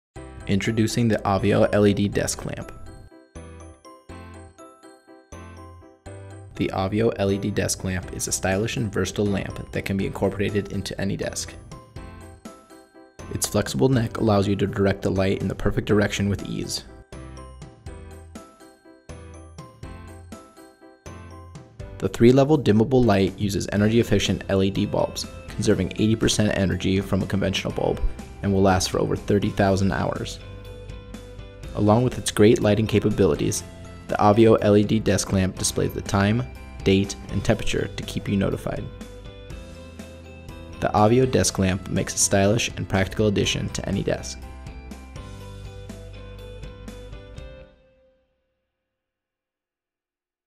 AVVIO LED Desk Lamp
High sensitivity dimmer allows for one-touch brightness adjustment (3 levels) - dimmable eye protective light
• Eco-friendly, long-lasting LED bulbs conserve nearly 80% energy, lasting over 30,000 hours
• Flexible neck allows for simple angle modification, great for desks, tables, offices, schools and college!
• LCD screen displays time, date, and temperature to keep you notified
• Plug this LED lamp with the included USB cable into your computer, or with the ETL certified 100-240V wall plug